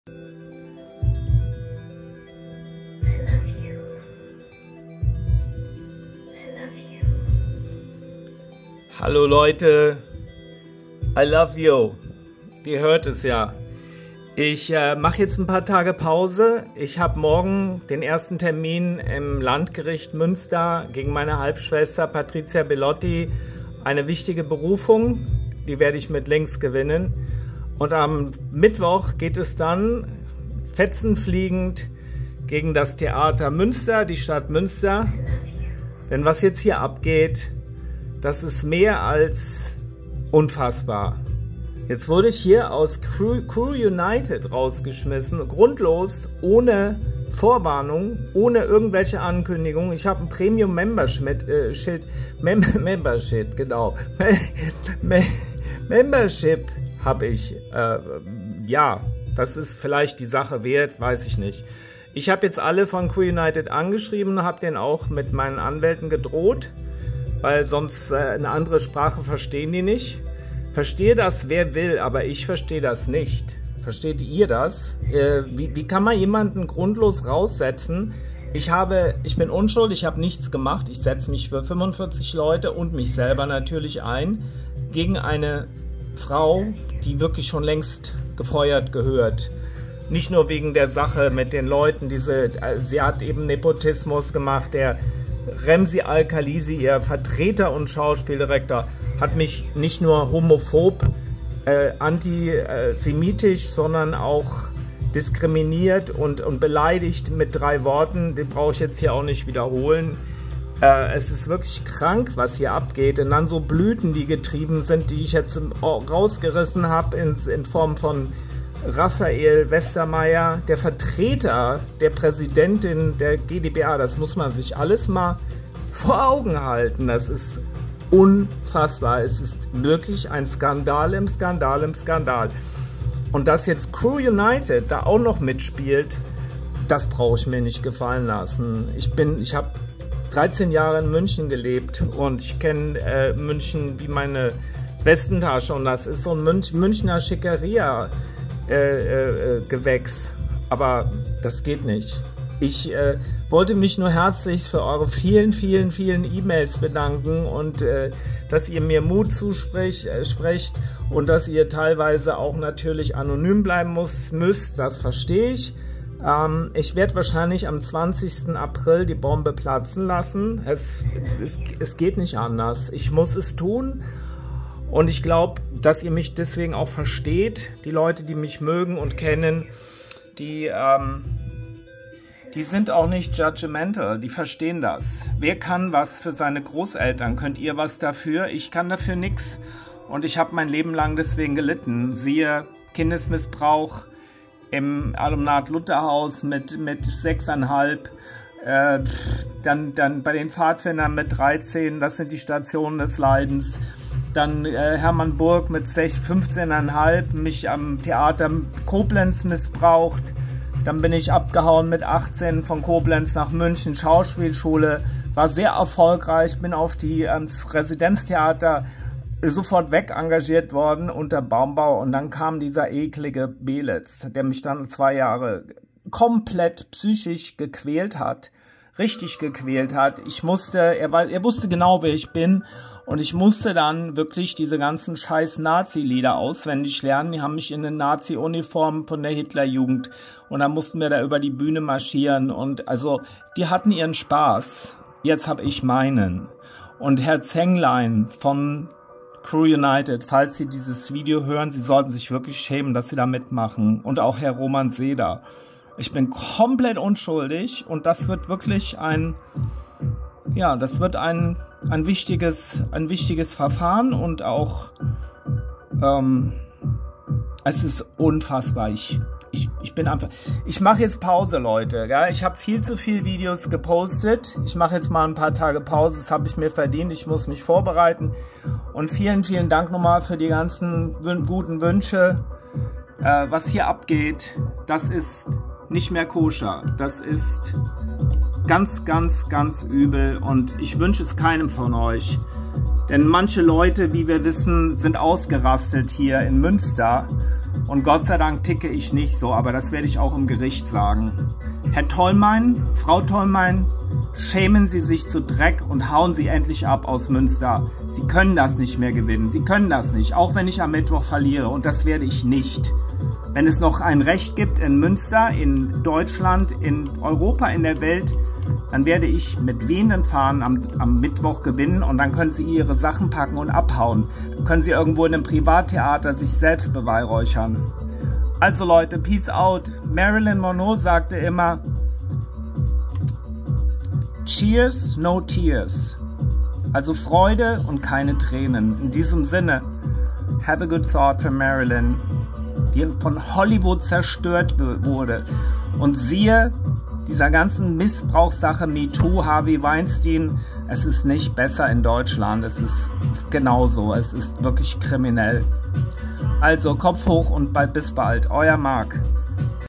DANKE! & CHEERS NO TEARS WÜNSCHT EUER INFLUENCER MARK BELLINGHAUS - BITTE DAUMEN DRÜCKEN! ;-)
ANSAGE an Oliver Zenglein, Roman Seda u. Mitarbeiter*innen,
ich konnte heute nicht in mein eigenes PREMIUM Abo von Eurer Webseite crew united, einloggen.
Was Ihr hier ohne VORWARNUNG getan habt, passt nach Russland, China oder Nordkorea, aber keinesfalls in die demokratische Bundesrepublik Deutschland. Sollten die üblen Verleumdungen eines Ehemanns namens Prof. Dr. Oliver Tolmein hinter dieser Aktion stehen, so habt Ihr, das Team von crew united Euch hiermit mitschuldig gemacht. Denn Ihr habt weder angefragt warum ich Videos über den größten Theaterskandal in der langen Geschichte Münsters postete, noch habe ich mich sonst irgendwie gegen die Vorschriften Eurer Webseite verhalten. Meine PREMIUM Mitgliedschaft, die sowieso teurer ist als die normale, lässt eine solch rechtswidrige Aktion Eurerseits auch überhaupt nicht zu!
Ich gebe Euch bis Mittwoch, den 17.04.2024 um mein sehr hart erarbeitetes Profil mit all meinen Arbeitsinfos, die Ihr somit gelöscht habt, wieder einwandfrei und in vorherigem Zustand wieder zu posten.
Was hier abgeht, ist nicht mehr kosher, und es ist wirklich ein um sich greifender KUNST-SKANDAL, der nicht nur den Bereich der Schauspielerei, sondern vor allem den Machtmissbrauch von Intendanten und sogar das Thema metoo berührt, anspricht, und sich durch Beweise damit auseinandersetzt.
Ich fordere hiermit Herrn Zenglein und seinen Partner Herrn Seda auf, mich weder zu diskriminieren, noch mich grundlos zu löschen, denn dies geschah OHNE VORWARNUNG, noch mit irgendeiner anfechtbaren Erklärung. ERGO: hier liegt ein eklatanter Rechtsbruch vor, der auf DISKRIMINIERUNG, VORURTEILEN und puerer HETZE, basiert!
Daher behalte ich mir vor eine Klage gegen crew united bei der STA München mit sofortiger Wirkung einzureichen.
Münster, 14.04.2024 Mark Bellinghaus (seit Jahren zahlendes Mitglied auf der Schauspieler/Künstler-Webseite crew united.

Artikel 3, Absatz 3 des Grundgesetzes der Bundesrepublik Deutschland besagt:

(3) Niemand darf wegen seines Geschlechtes, seiner ABSTAMMUNG, seiner Rasse, seiner Sprache, seiner Heimat und Herkunft, seines Glaubens, seiner religiösen oder politischen Anschauungen benachteiligt oder bevorzugt werden.

Theater der Stadt Koblenz, Ort des Missbrauchs, als ich 15 Jahre alt war, und in diesem Theater als Statist arbeitete und erste Schritte in Richtung Schauspielkarriere unternahm. Ich fragte den allseits beliebten, respektierten und so allwissenden Kammerschauspieler Hermann Burck, ob er auch unterrichten würde, da ich parallel in einem Volkshochschulkurs über Schauspielerei teilnahm.
Das waren meine allerersten professionellen Schritte in Richtung meines Traum-Berufes: Schauspieler!
Aber warum ist das für die Redaktion der Rhein-Zeitung Koblenz nicht nachzuvollziehen, dass ich die Öffentlichkeit über meinen schlimmen Fall von METOO u. sexuellem Missbrauch an und mit Schutzbefohlenen berichten muss. Nicht möchte, sondern muss. Denn ich bin mir sicher, dass dieser alte Kerl das mit Jungs die noch nicht volljährig waren, so wie ich damals, bis in sein hohes Alter getrieben hat! Das ist meine Überzeugung, denn ich bin in meinem Leben leider an drei weitere schlimme Verbrecher und Kerle geraten, die sich genauso wie Herr Hermann Burck mir gegenüber verhielten.
Günther Beelitz, der Ex-Chef einer perfiden Katharina Kost-Tolmein, die schon bald aus ihrem Amt als noch amtierende Generalintendantin der Stadt Münster fliegen muss und wird, will diese Stadt in der ich seit 14 Jahre wohne u. arbeite glaubhaft bleiben.
Bernd Eichinger, der Mega Filmproduzent, der mich bei meinem ersten Vorstellungsgespräch mit seiner Droge nach Wahl, nämlich Koks, in Berührung gebracht hat. Der Leute wie Hannes Jaenicke und Til Schweiger zu seinen Betthasen machte, bei mir jedoch auf Granit stieß, weil ich in einer festen Beziehung stand und all diese sexuellen Übergriffe meinem Freund Walter sofort erzählte. Mein Partner und Freund wollte damals den widerlichen Bernd Eichinger zur Rede stellen und ihm aufs Maul hauen.
Außerdem waren da noch der Bernd Eichinger Freund Horst D. Scheel, der mich zu einem Casting für die f*cking "Lindenstraße" nach Köln einlud, und voller Verwunderung stand ich in Herrn Scheels PRIVATWOHNUNG. Die Casting Couch wurde hier zu seinem Bett umfunktioniert.
Nein, was damals (teilweise wie auch heute) in Bezug auf metoo abgeht, ist mehr als krank. Zumal man weiß, dass mit Harvey Weinstein ein Täter in den USA richtig fest und zu Recht im Knast schmort. Hier ist es so, als müsse ich mich schämen, weil ich mich bei Eichinger für "Der Name der Rose" vorgestellt hatte. Und weil dieser Irre und Adolf Hitler-Fanatiker über mich herfiel, als wäre ich essbar!
Ich fordere Til Schweiger und Hannes Jaenicke hiermit auf, ihr jahrzehntelanges Schweigen endlich, endlich zu brechen!